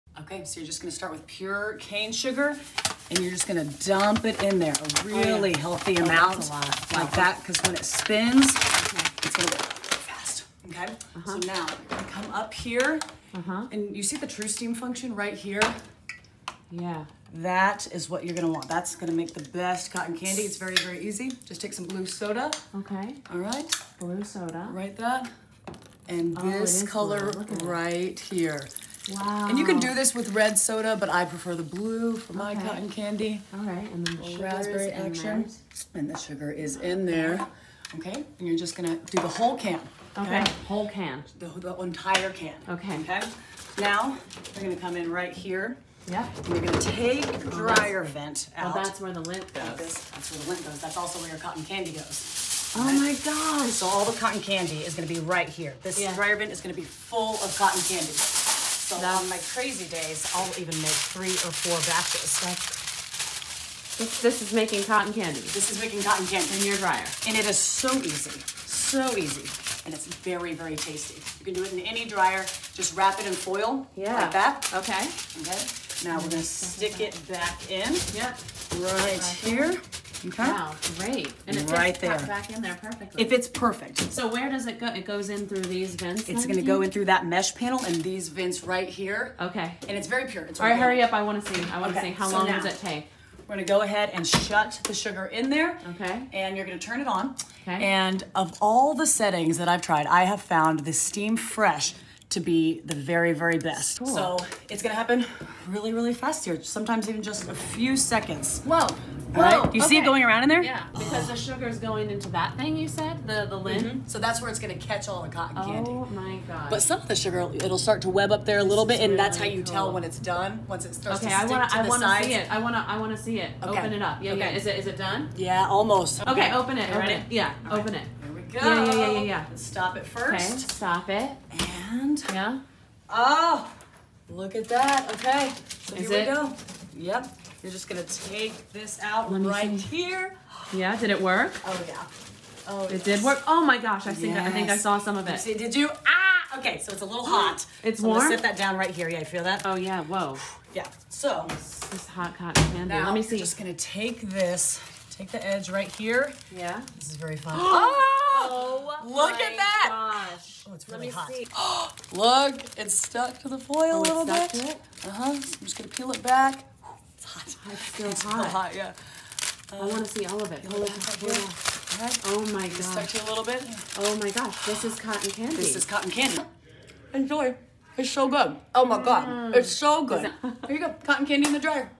How To Make Cotton Candy in the Dryer (Parody)
Making cotton candy in the dryer is indeed possible 😂